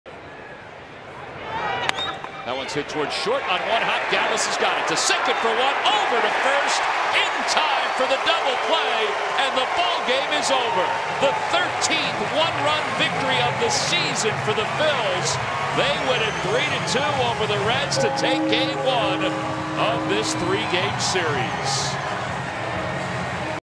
CIN@PHI: Gomez induces double play to earn the save
Jeanmar Gomez gets Brandon Phillips to hit into a 6-4-3 double play, earning the save in the Phillies' 3-2 win